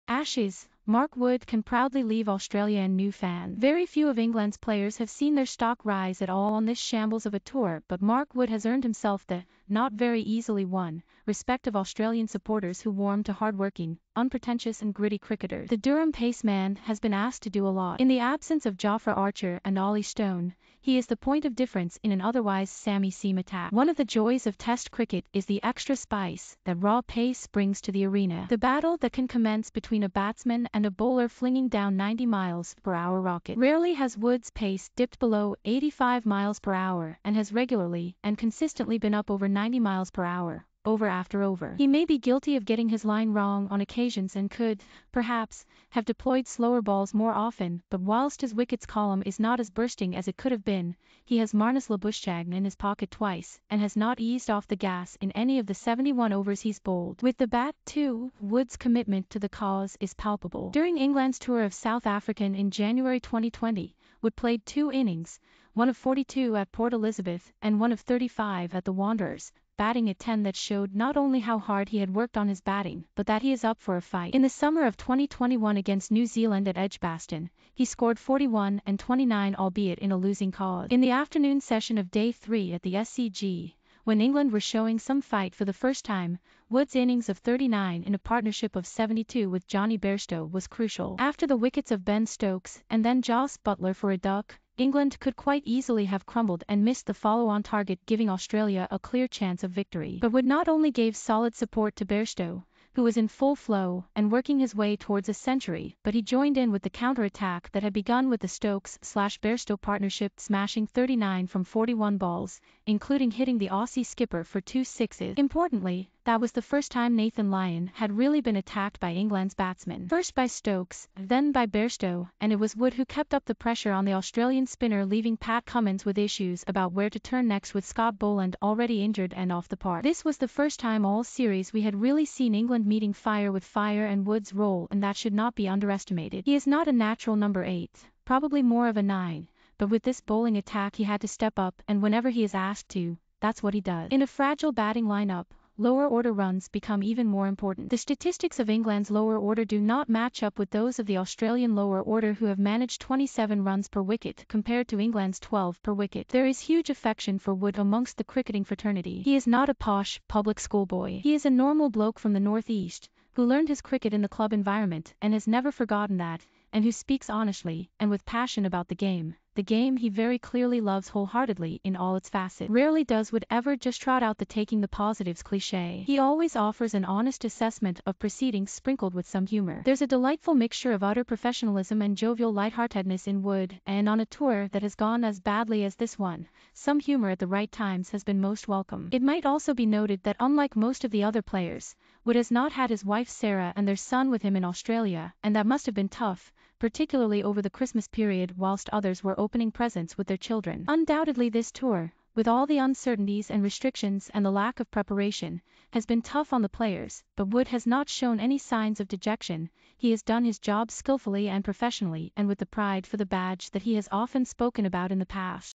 Ashes: Mark Wood can proudly leave Australia-and new fans
Wood’s innings of 39 in a partnership of 72 with Johnny Bairstow was crucial. After the wickets of Ben Stokes and then Jos Buttler for a duck Mark Wood can proudly leave Australia-and new fans Very few of England’s players have seen their stock rise at all on this shambles of a tour but Mark Wood has earned himself the that’s what he does. In a fragile batting line up then by Bairstow and it was Wood who kept up the pressure on the Australian spinner leaving Pat Cummins with issues about where to turn next with Scott Boland already injured and off the park. This was the first time all series we had really seen England meeting fire with fire and Wood’s role in that should not be underestimated. He is not a natural number eight Wood’s commitment to the cause is palpable. During England’s tour of South African in January 2020 lower order runs become even more important. The statistics of England’s lower order do not match up with those of the Australian lower order  who have managed 27 runs per wicket compared to England’s 12 per wicket. There is huge affection for Wood amongst the cricketing fraternity. He is not a posh public schoolboy. He is a normal bloke from the north east England could quite easily have crumbled and missed the follow-on target giving Australia a clear chance of victory. But Wood not only gave solid support to Bairstow cliché. He always offers an honest assessment of proceedings sprinkled with some humour. There’s a delightful mixture of utter professionalism and jovial light-heartedness in Wood and on a tour that has gone as badly as this one he is the point of difference in an otherwise samey seam attack. One of the joys of Test cricket is the extra spice that raw pace brings to the arena. The battle that can commence between a batsman and a bowler flinging down 90mph rockets. Rarely has Wood’s pace dipped below 85mph and has regularly and consistently been up over 90mph – over after over. He may be guilty of getting his line wrong on occasions and could that was the first time Nathan Lyon had really been attacked by England’s batsmen. First by Stokes batting at 10 that showed not only how hard he had worked on his batting but that he is up for a fight. In the summer of 2021 against New Zealand at Edgbaston has been tough on the players but Wood has not shown any signs of dejection